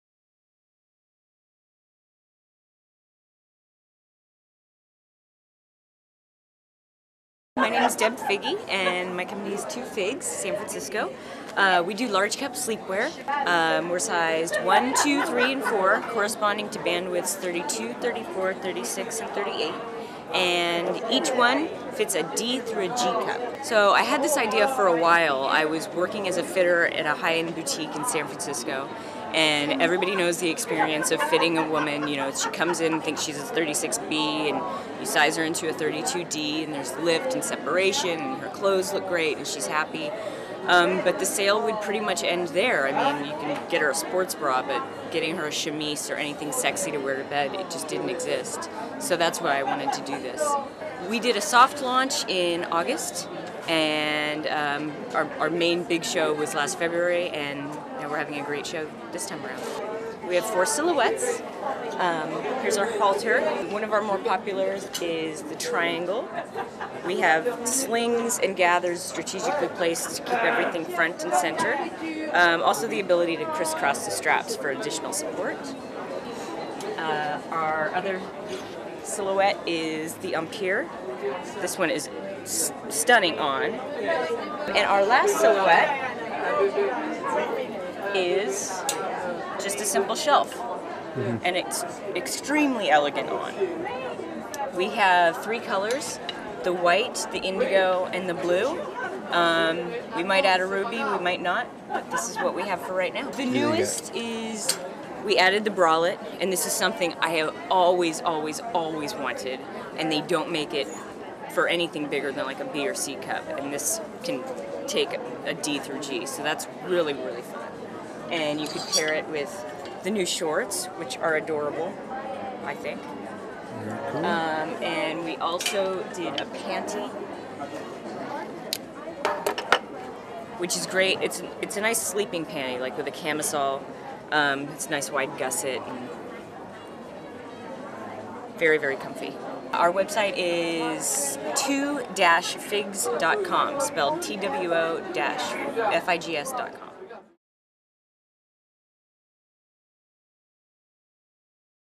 Two Figs at CurveNY August 2011
Debra Figge of Two Figs talks about her line Two Figs an elegant sleepwear line for designed to accommodate D-G cups.  For more information on Two Figs go